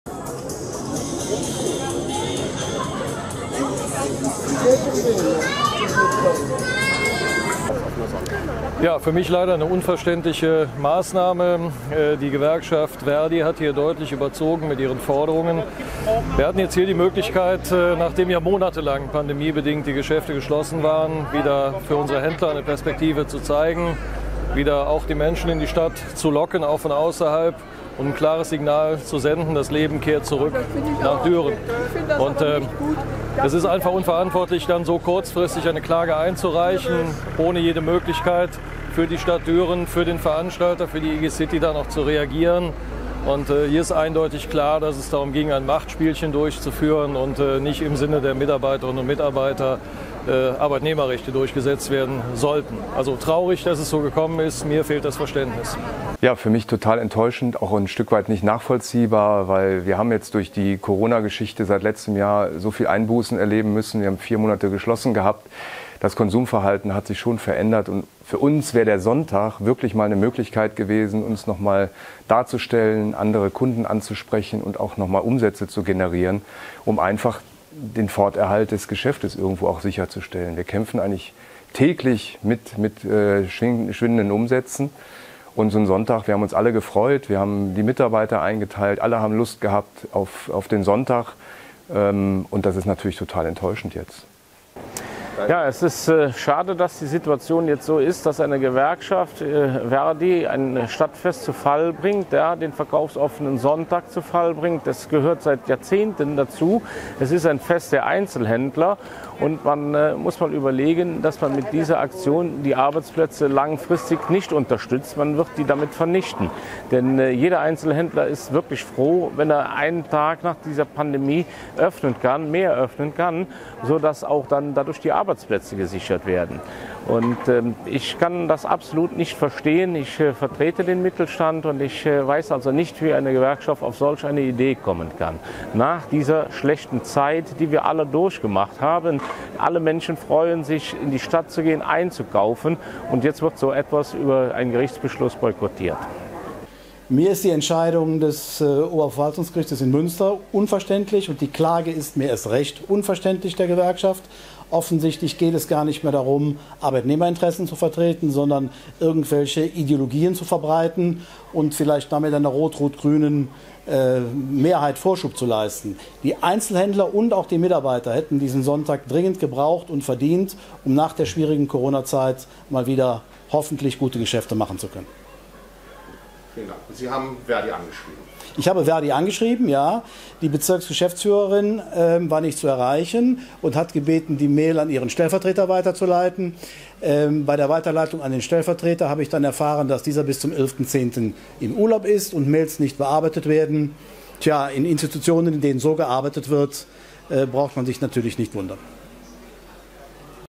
Verdi zerstört Verkaufsoffenen Sonntag - Meinungen aus Düren
Der für den morgigen Sonntag geplante Verkaufsoffene Sonntag wurde von der Gewerkschaft ver.di zerstört. DN-News.de hat einige Statements aus Düren eingefangen.